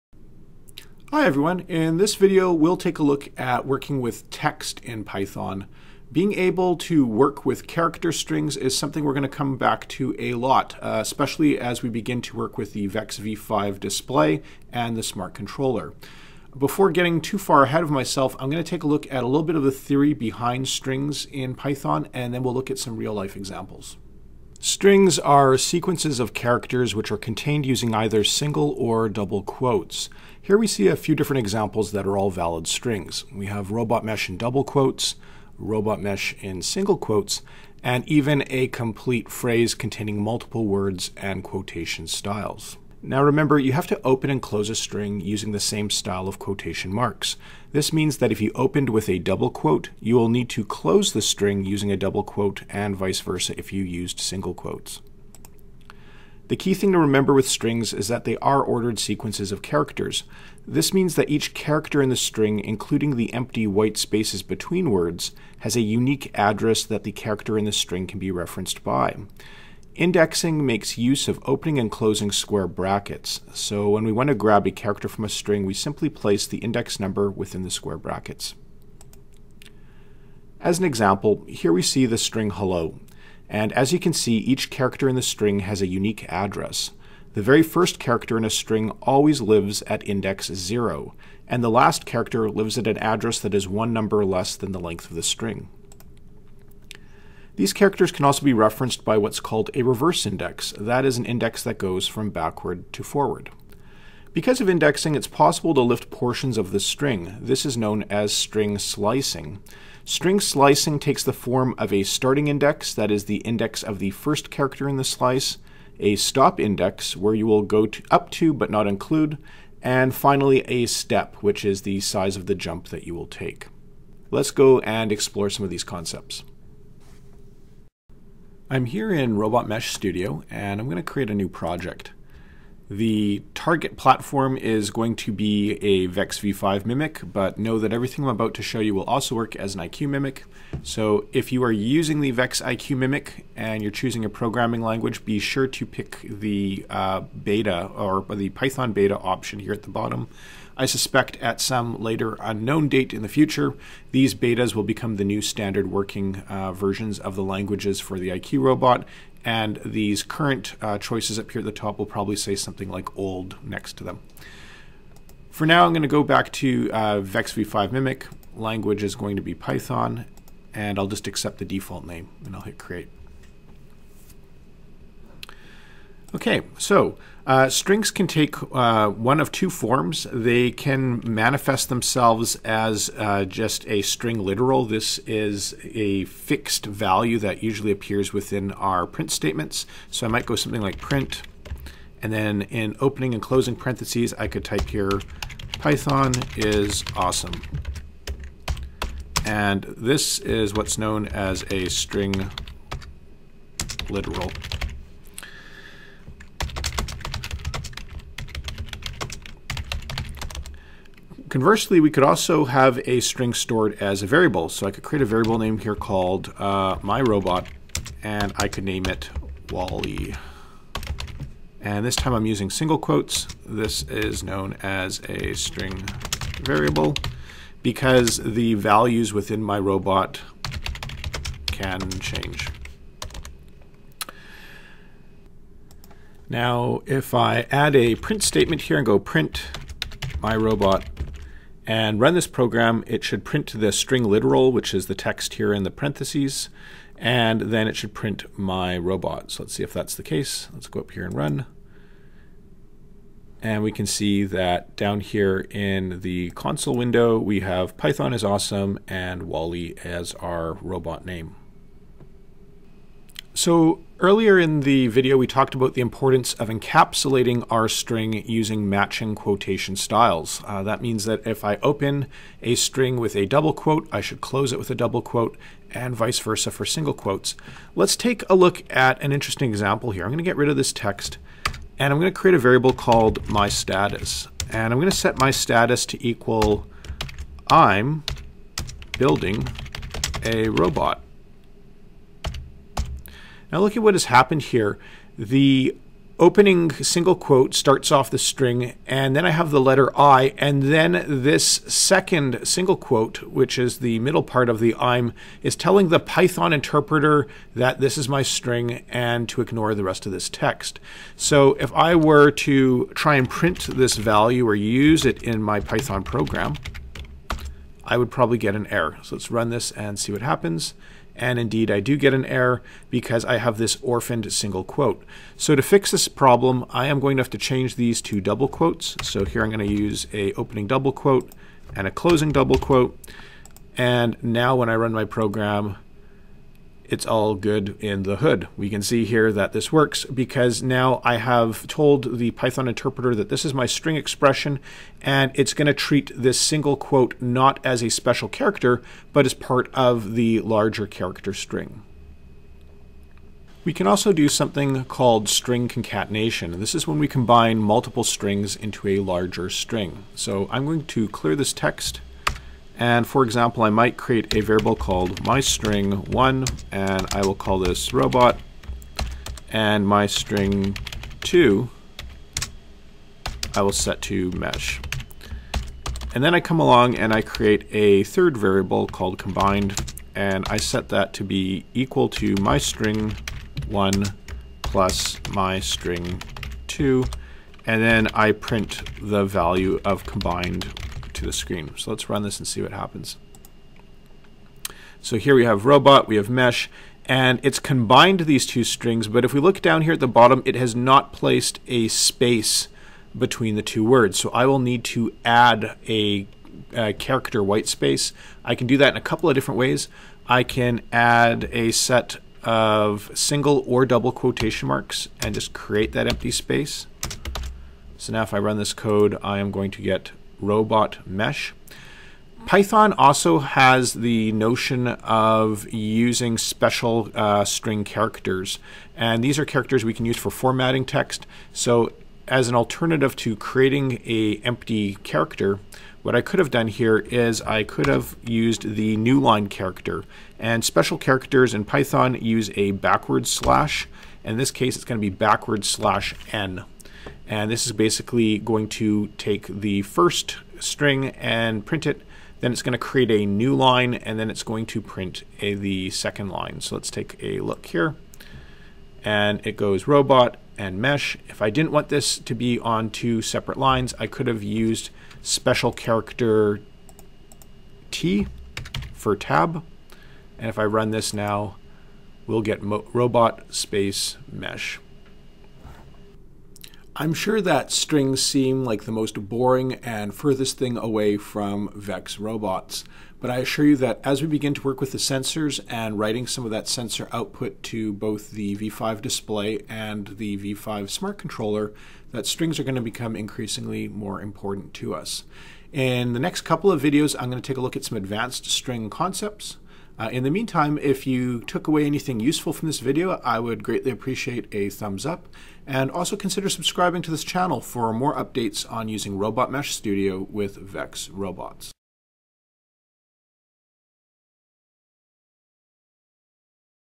Working with Character Strings in Python using Robot Mesh Studio
Strings are an ordered sequence of characters. We'll be working with Strings quite a bit as we begin to write to the V5 Display and the small LCD on the Smart Controller.

The software I am using is Robot Mesh Studio, a free, tournament-legal, Chrome-browser extension that supports Python programming (among other languages) for both VEX IQ and VEX EDR (V5 + Cortex).